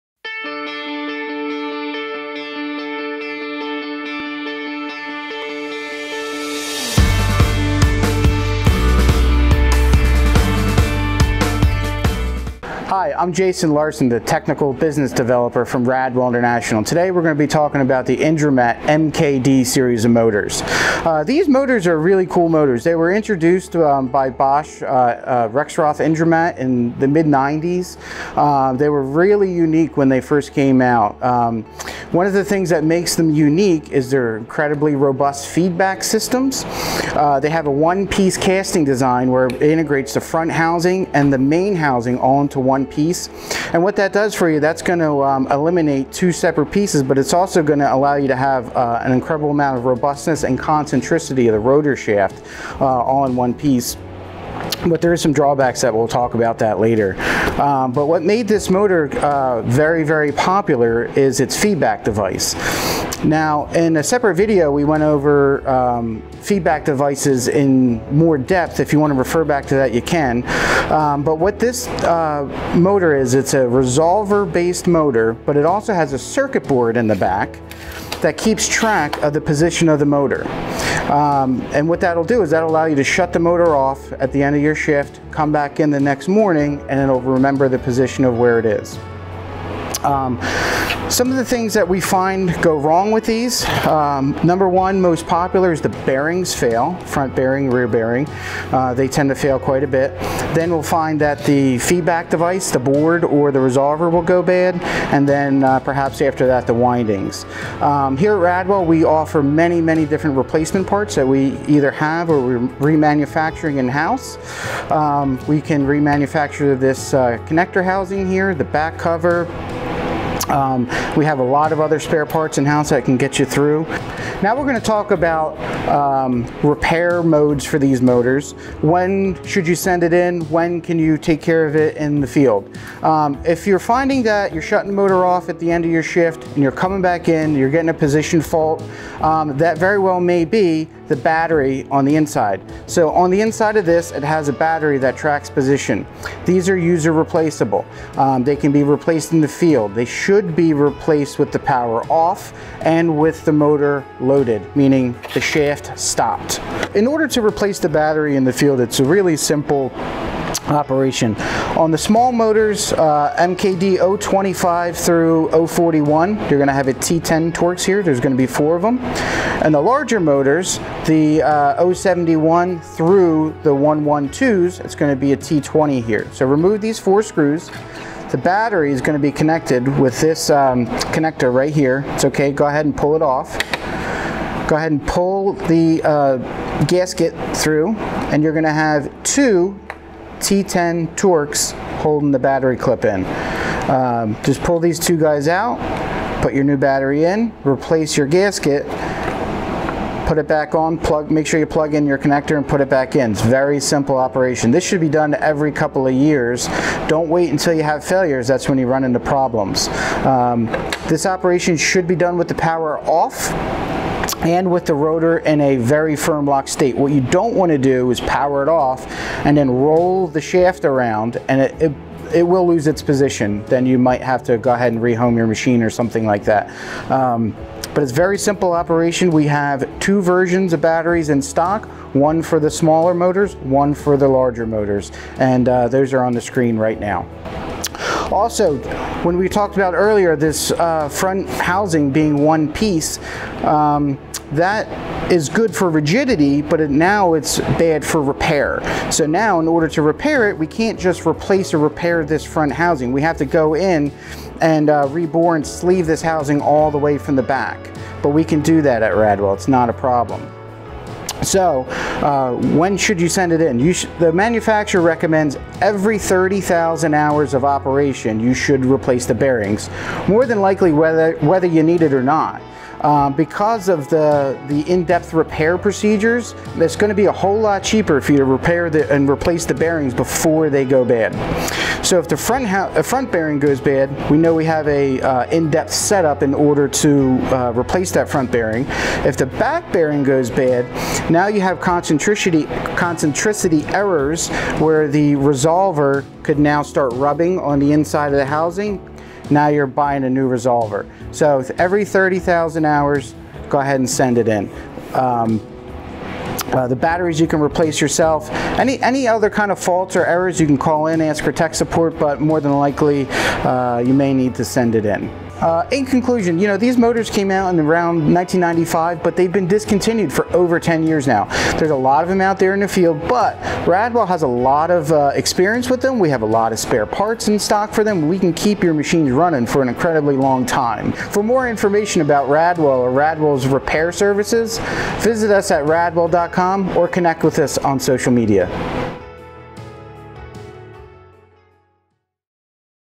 Indramat MKD Series Repair Overview
In this video from Radwell TV, get a repair overview of the Indramat MKD Series

As mentioned in the video, learn more about Servo Motor Repair in this two part tour of our Motor Shop at Radwell Headquarters in Willingboro, NJ

To order the replacement batteries mentioned in this video:
Replacement Batteries for MKD series (025, 041, 071) =R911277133-SUB

At Radwell TV, we leverage our 40+ years of industry experience to bring you the best in industry information and education.